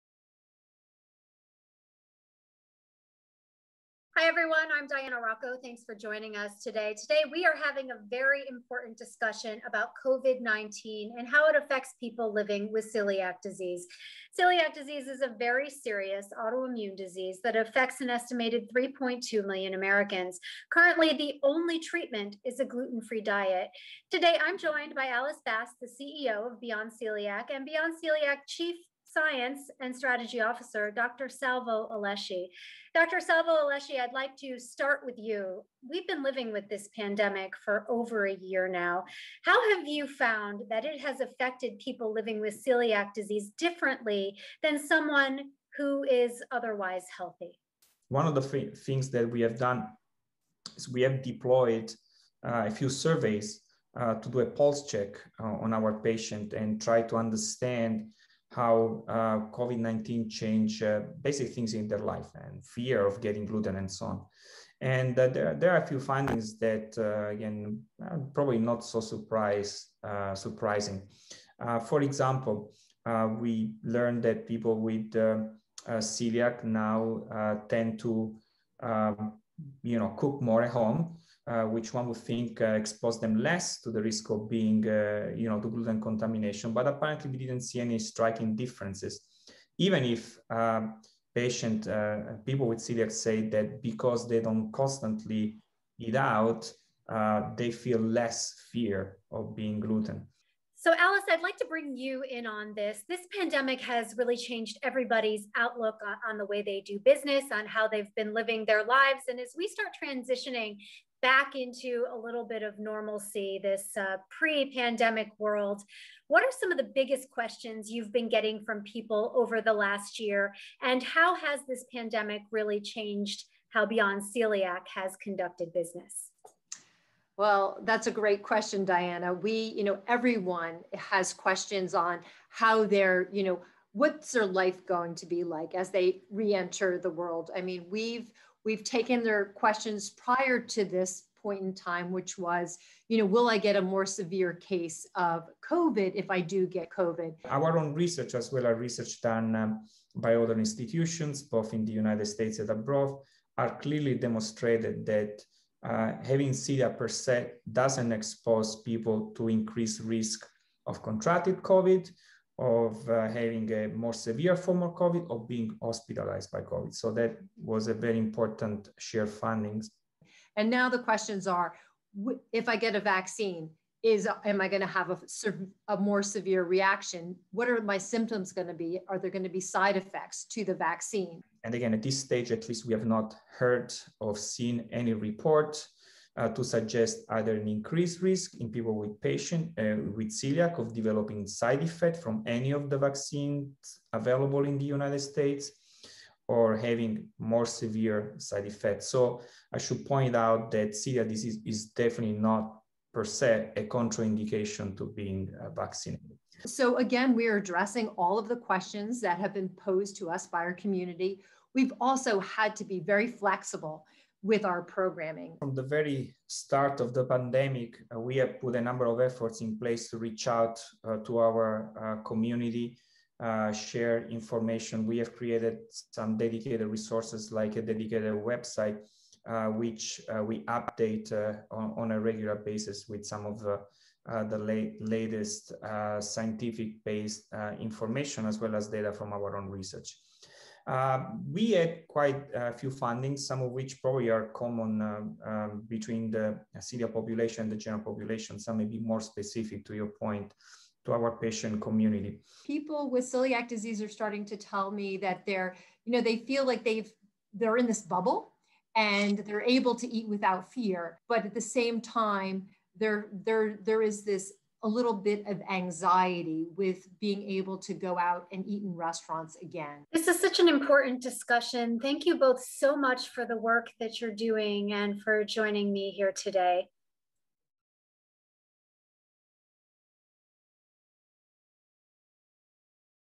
Celiac Disease and COVID-19
Diana Rocco speaks with Beyond Celiac CEO Alice Bast and Beyond Celiac Chief Scientist and Strategy Officer, Salvatore Alesci, MD, PhD, about celiac disease and COVID-19.

Get all the latest information and a roundup of available research on celiac disease and COVID-19/novel coronavirus from Beyond Celiac